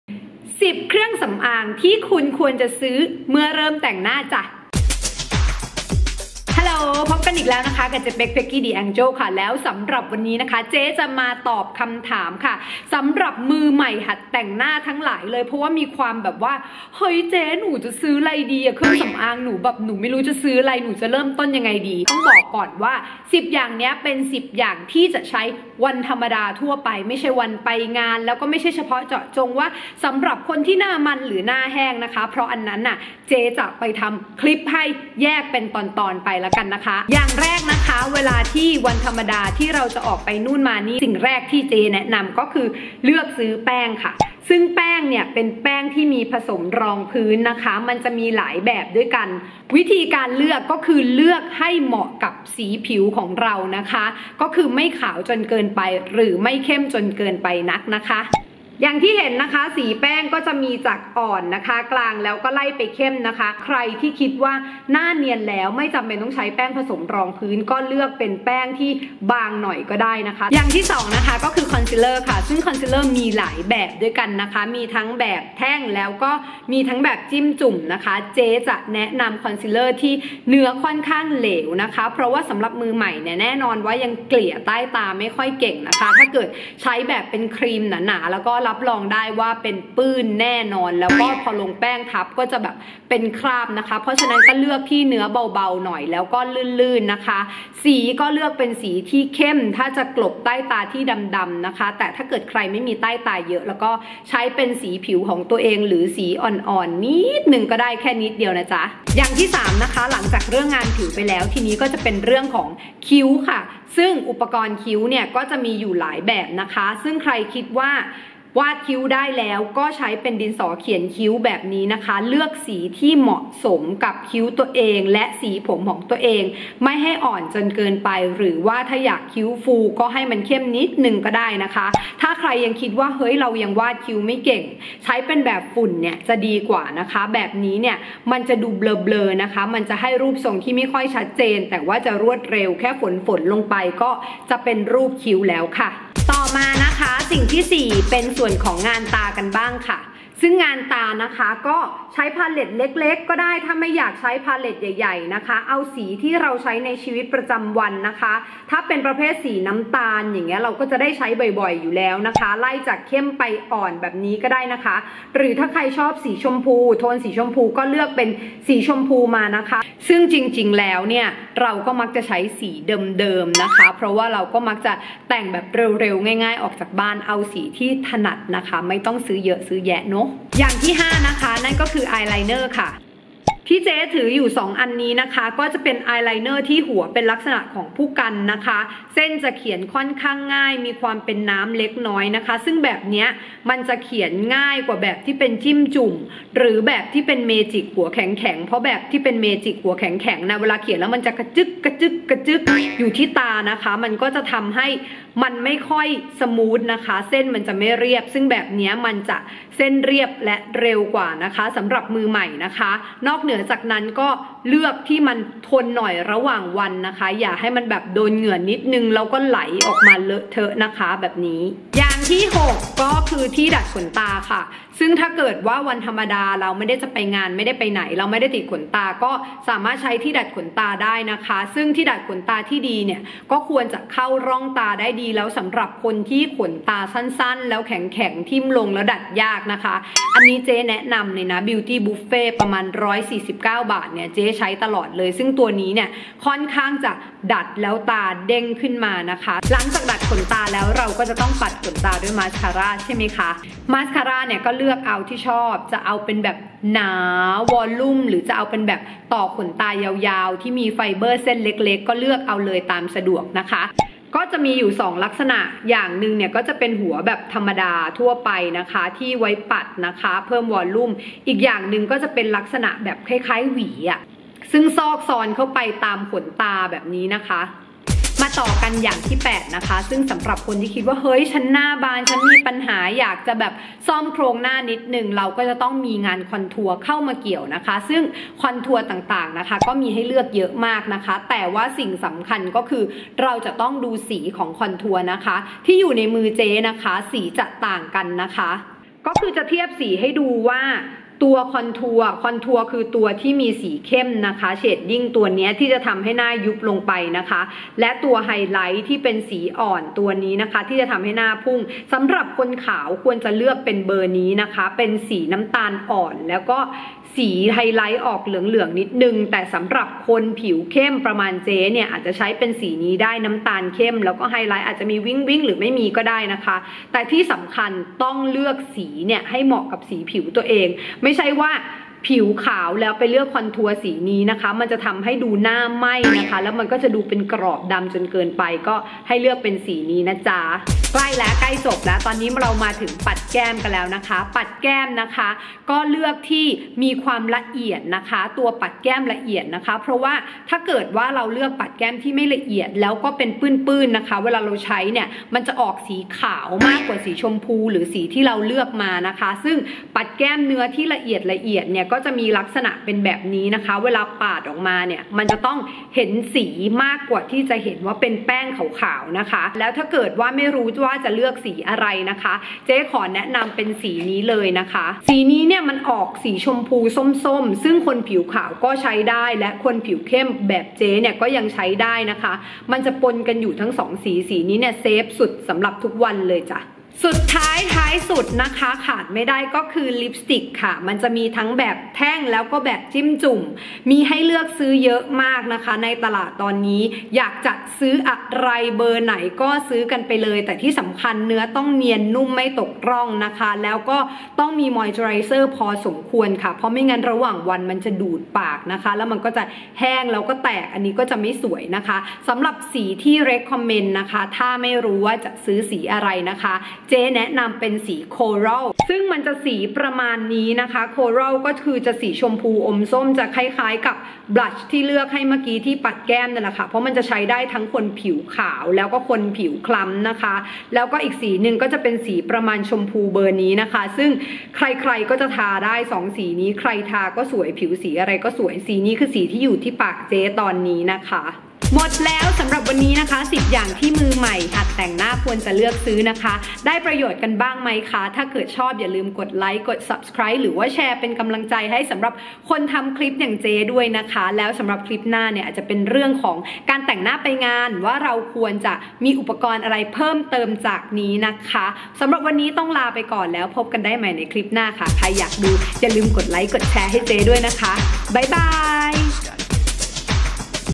การเลือกเครื่องสำอาง10 อย่างสำหรับมือใหม่หัดแต่ง | เจ๊เป็ก
มือใหม่หัดแต่งหน้า ไม่รู้จะเลือกซื้อเครื่องสำอางอะไรดี
เจ๊มีคำตอบ และวิธีเลือกซื้อมาฝากกันค่า